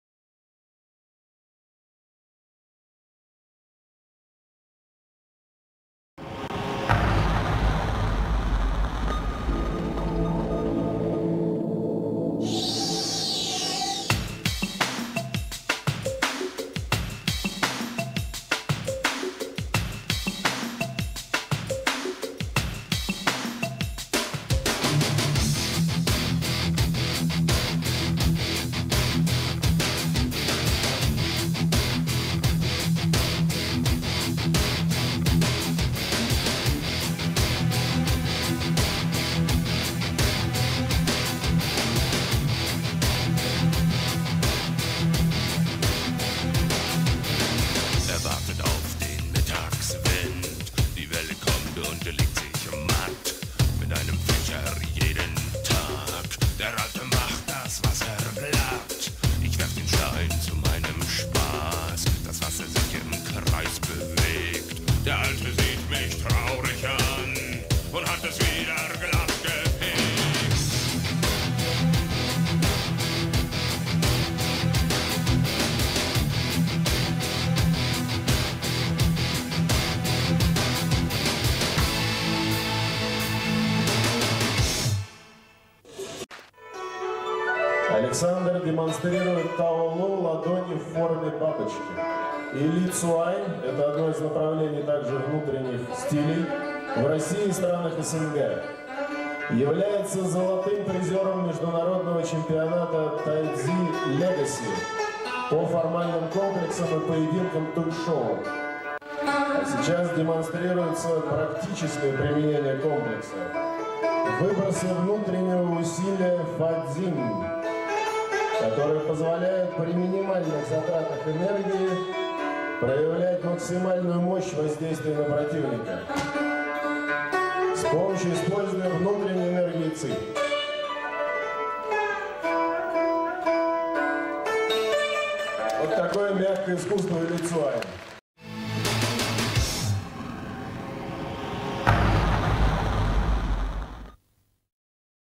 30 октября 2024 г.
Фестиваль Боевых Искусств на ТВ в Останкине.
Демонстрируют Александр Скалозуб и Андрей Чанг (Ендерюков).
А. Скалозуб демонстрирует комплекс с фацзинь Худечжан (Ладони в форме бабочки) и применение - юнфа.